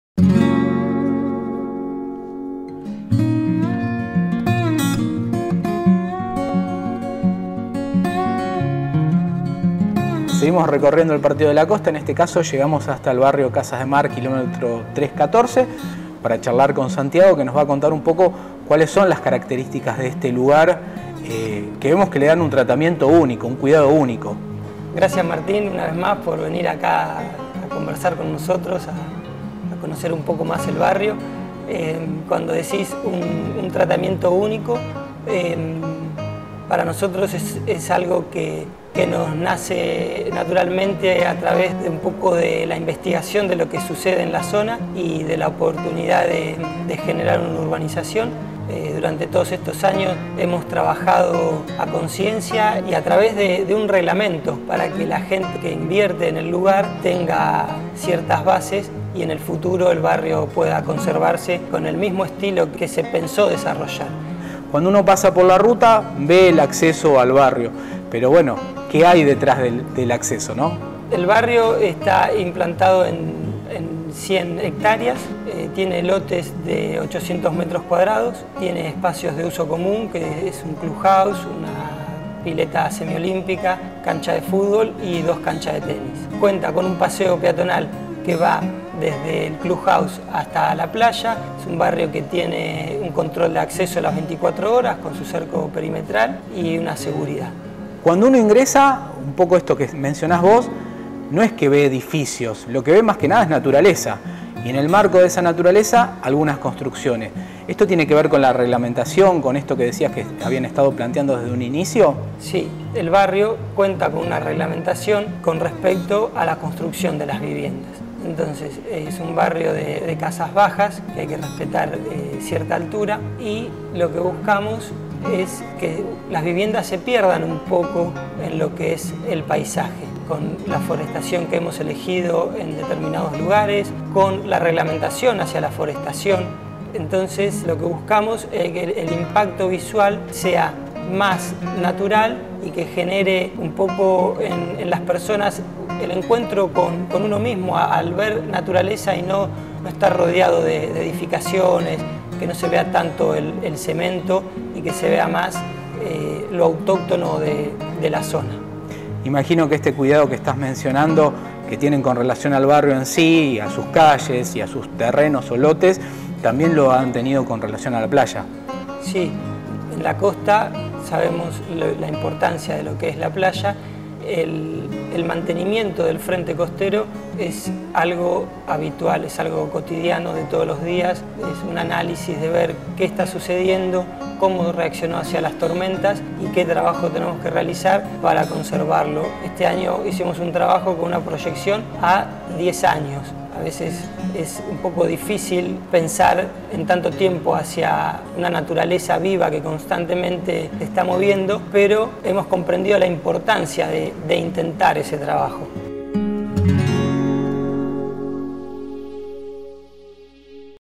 Km314, casas de mar en armonía con la naturaleza
El desarrollo urbanístico está pensado para conservar el entorno natural y brindar una experiencia a sus habitantes.

El barrio está implantado en 100 hectáreas, tiene lotes de 800 metros cuadrados, espacio de usos común como un Club House, pileta semi olímpica, canchas de fútbol y dos canchas de tenis. Además, cuenta con un paseo peatonal que va desde el Club House hasta la playa. Es un barrio que tiene control de acceso las 24 horas, con cerco perimetral y seguridad.